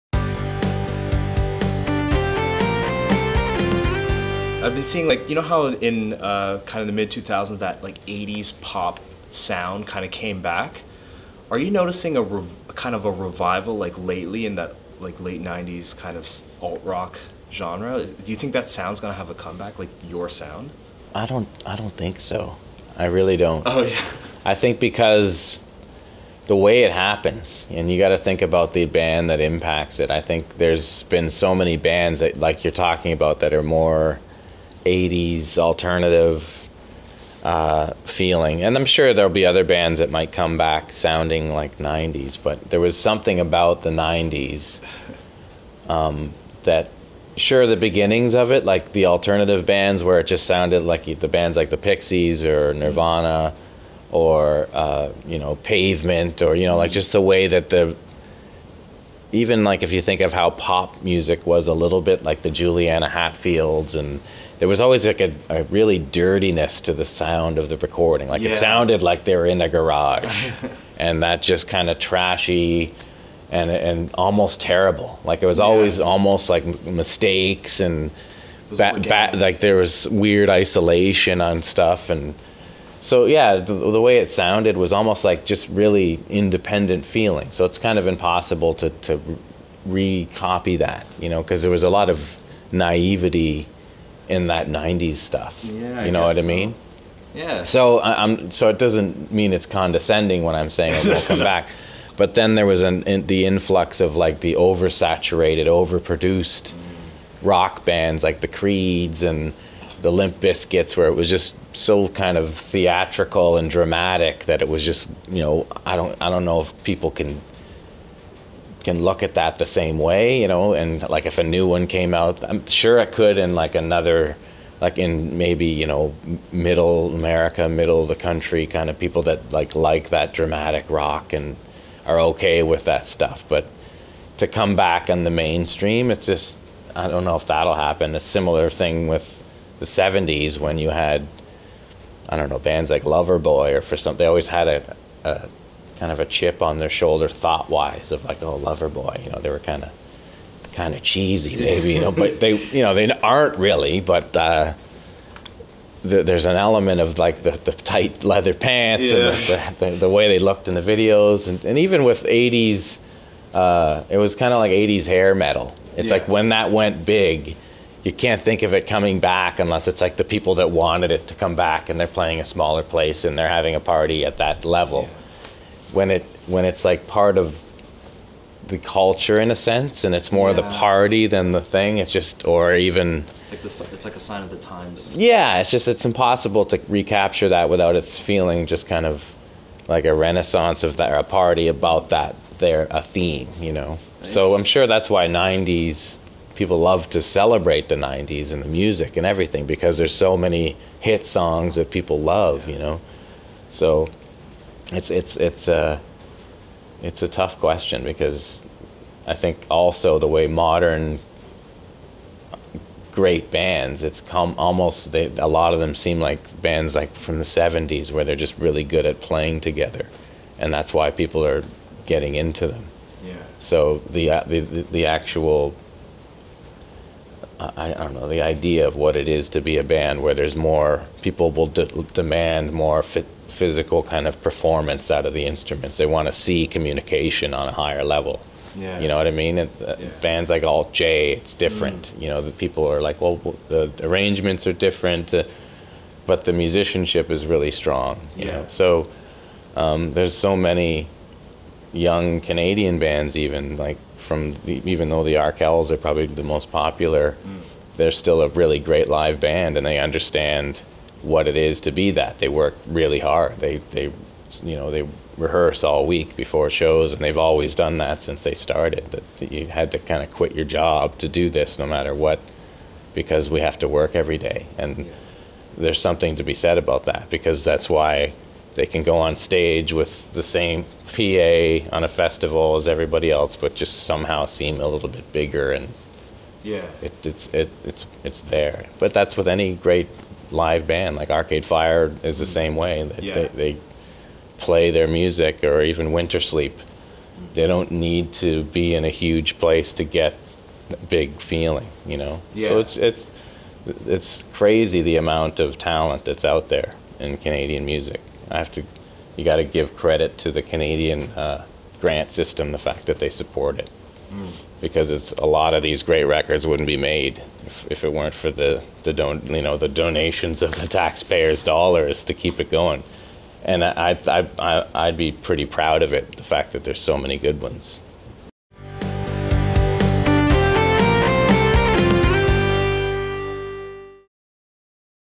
Jeremy Taggart Interview - On the sound of the '90s [Part 2]
Neil Shukla sat down with Jeremy Taggart at Cosmo Music to discuss Our Lady Peace, the sound of the 90s, his Taggart 'N' Torrens Podcast, and the upcoming drum clinic on September 25th in the Cosmopolitan Music Hall.